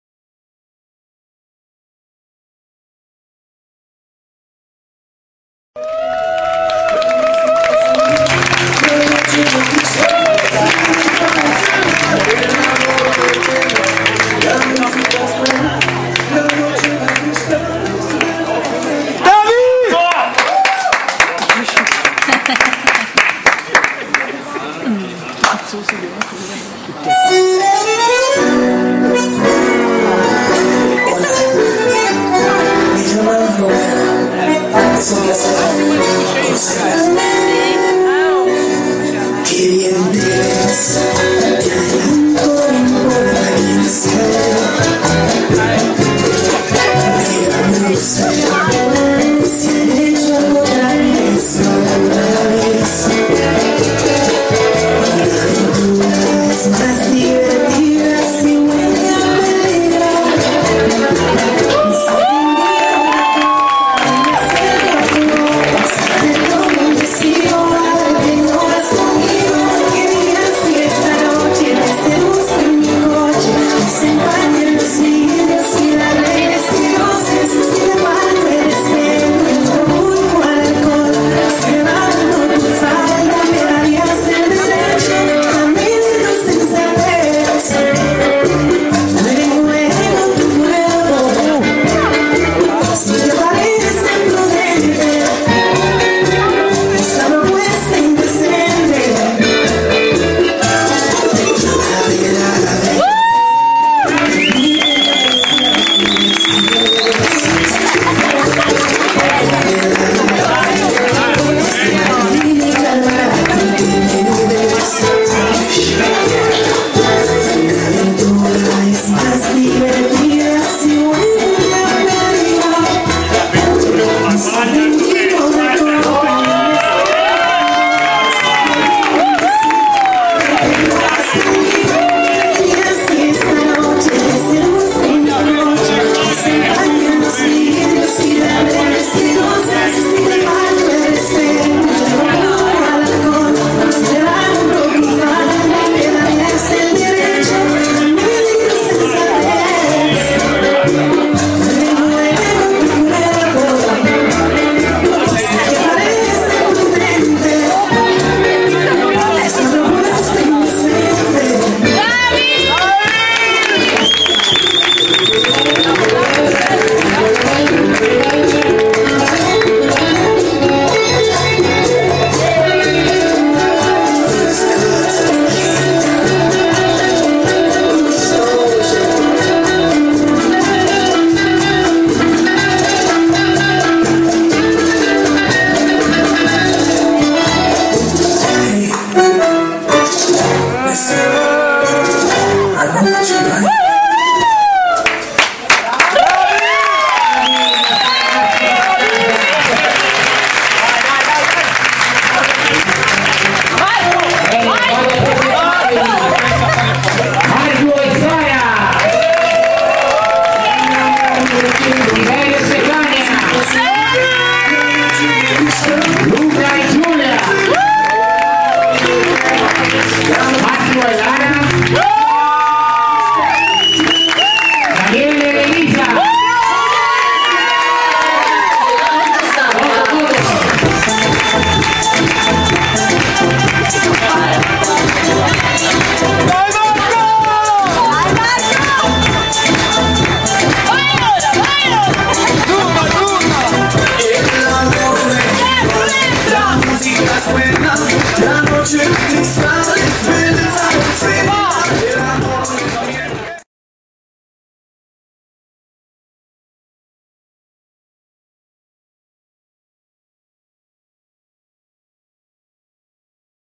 SAGGIO IN BLACK 22.06.2014 - BACHATA - SALSA IN BLACK
Saggio di fine anno accademico del corso di Bachata della scuola SALSA IN BLACK A.S.D.
Sede dei Corsi: Circolo ARCI di Cortenuova - Via del Ponte 22 - Empoli, Italy